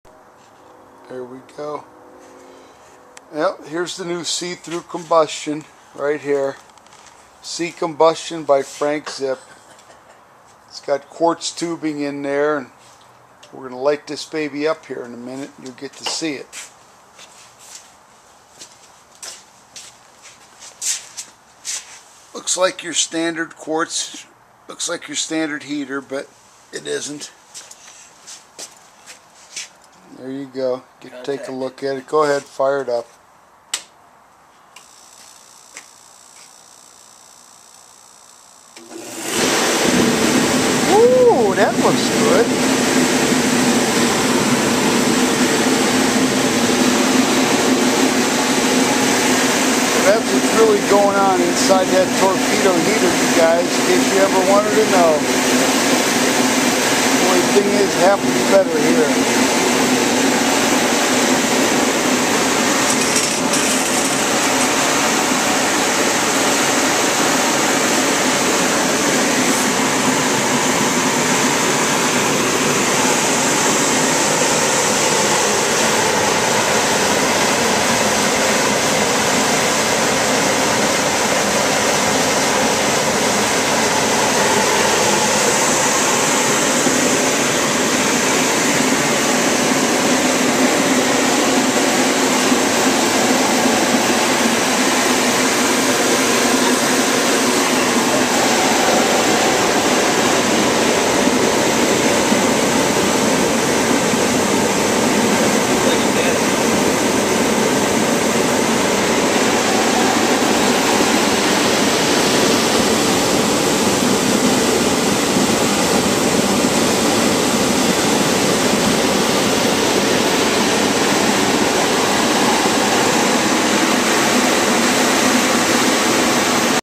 See thru combustion by Frank Zipp
Bullet heater modified with quartz to see actual combustion, also modified for high performance by Frank Zipp